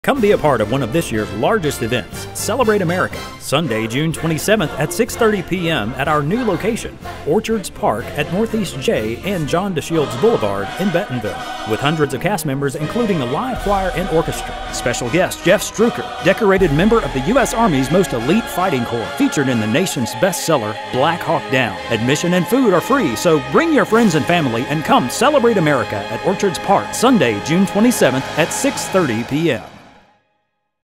Celebrate America TV Spot 2010
Celebrate America 2010 will be held at our new location at Orchards Park in Bentonville on Sunday June 27th at 6:30pm.